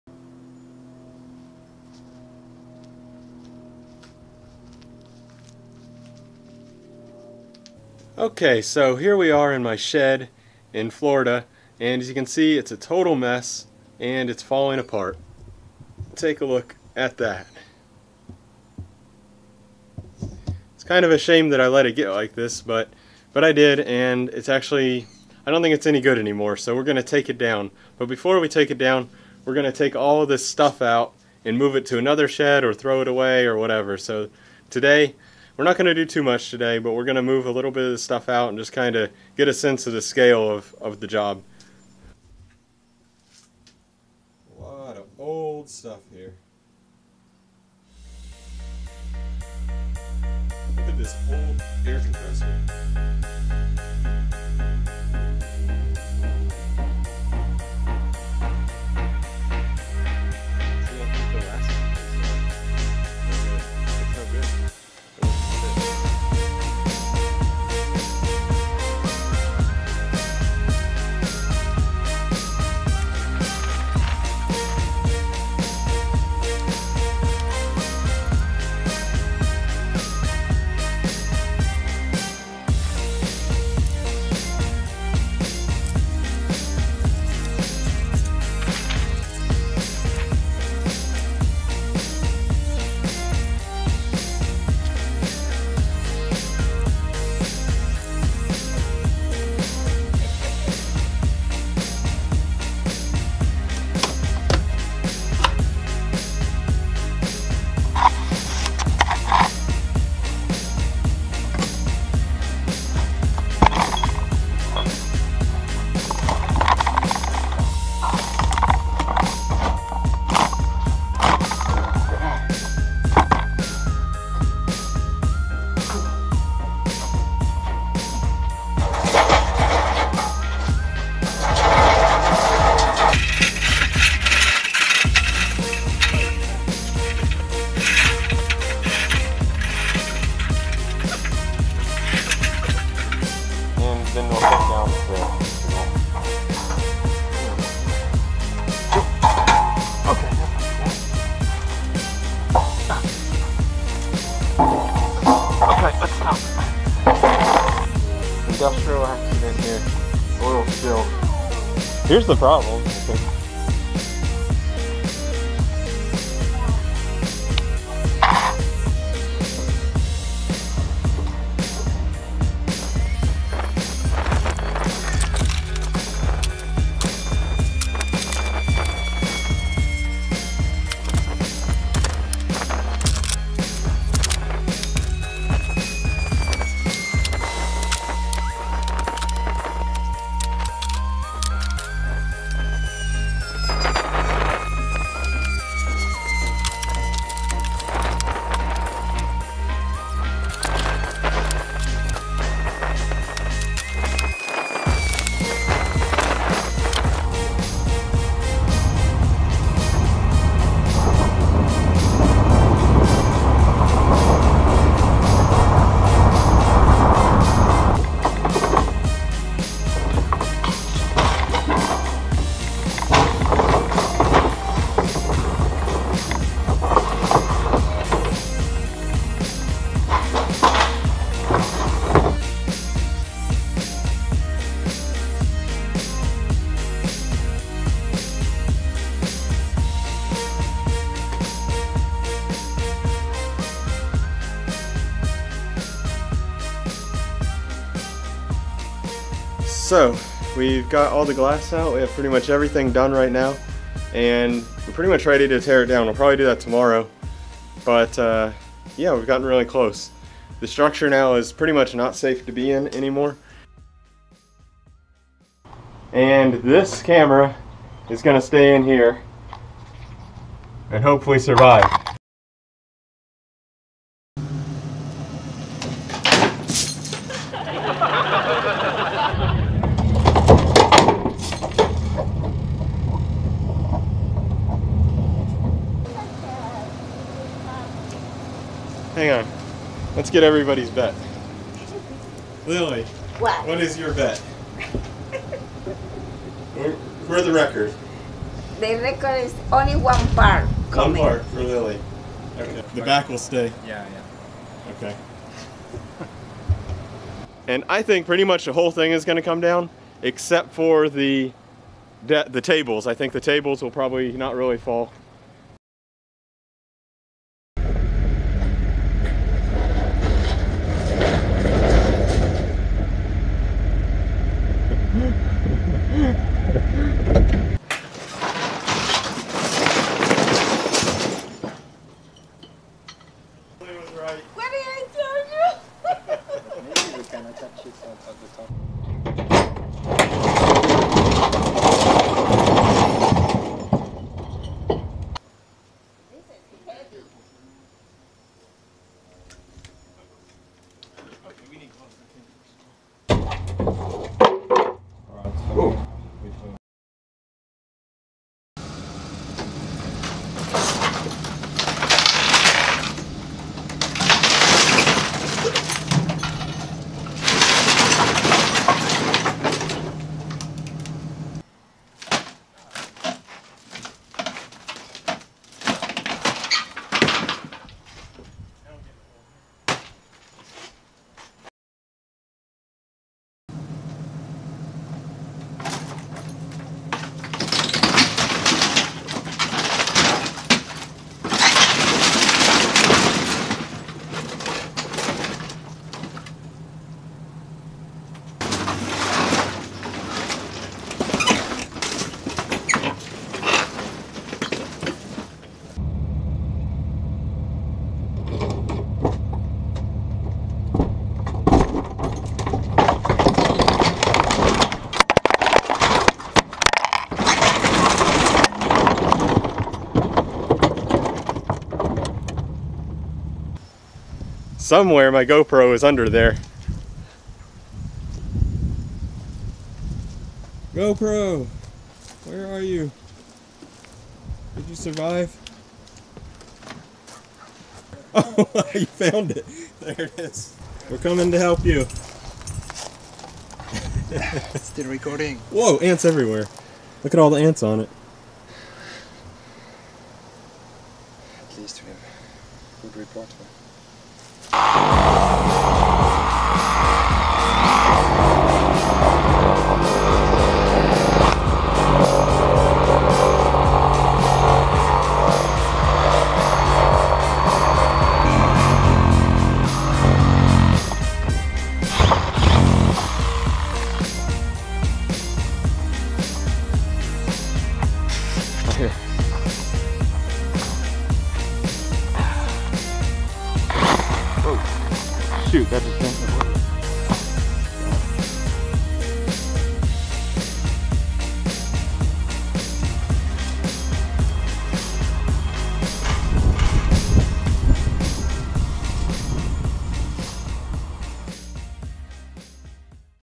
We took down my old shed
This shed was really old and falling apart. Watch as Oli and I pull it over with a truck.